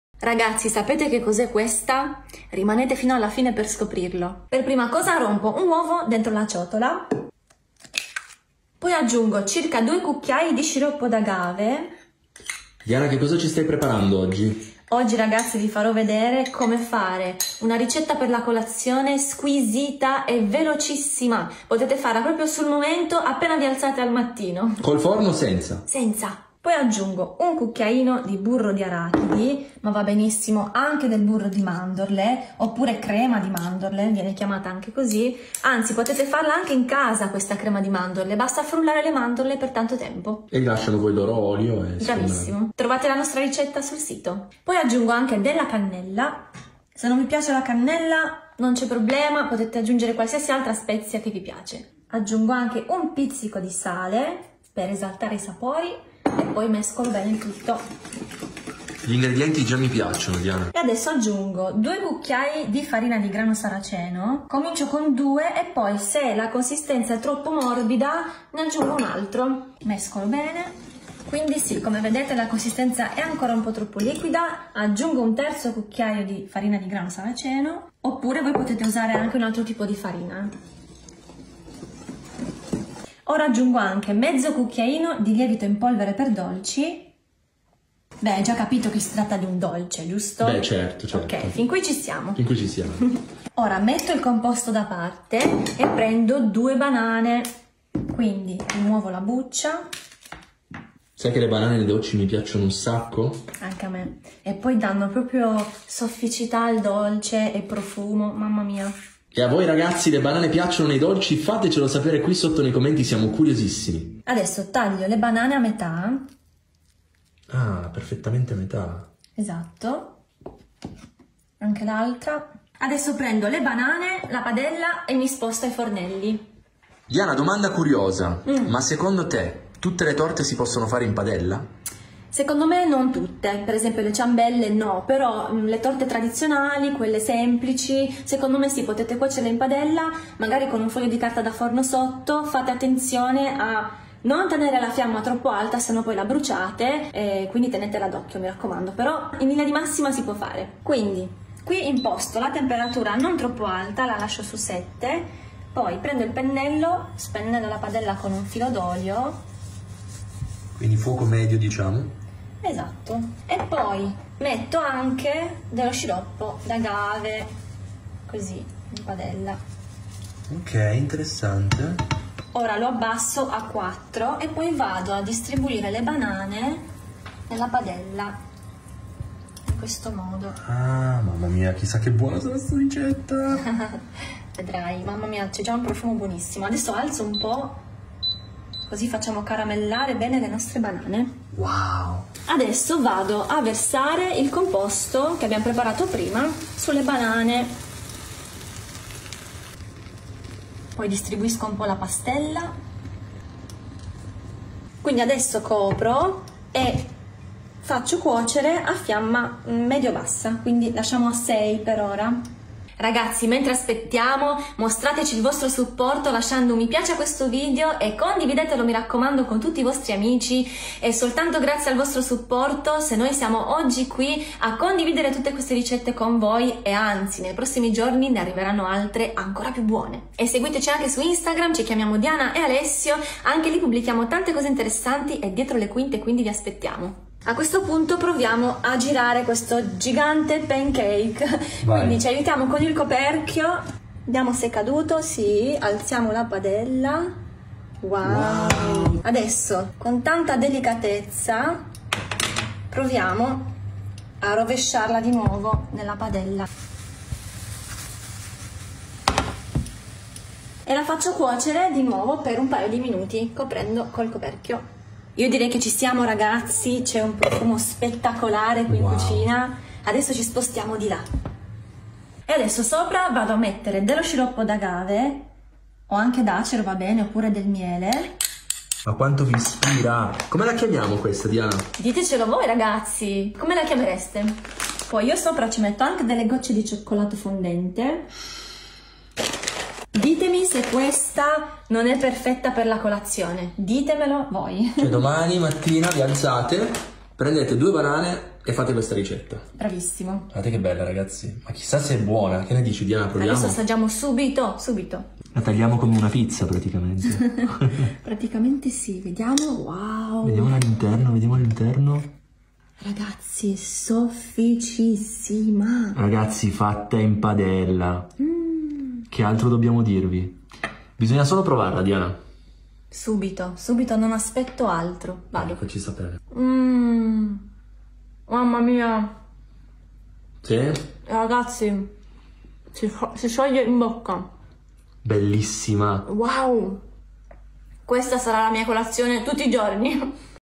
La TORTA in PADELLA che sta facendo impazzire tutti quanti! Fatta con 1 solo uovo
Wow! Finalmente una torta in padella facile e veloce fatta con pochi ingredienti!

INGREDIENTI
2 banane
3 cucchiai farina di grano saraceno
1 uovo
1 cucchiaino burro di arachidi
2 cucchiai sciroppo di agave
Mezzo cucchiaino lievito in polvere per dolci
Cannella q.b

Banana no gruppo A e AB, sostituire con mele o pere.
Farina di grano saraceno no gruppo B,AB, sostituire con un altro tipo di farina consentita.
Burro di arachidi no gruppo 0 e B sostituire con burro di mandorle.
Sciroppo d’agave no gruppo 0,B,AB sostituire con sciroppo d’acero
Cannella no gruppo 0 e B, ometterla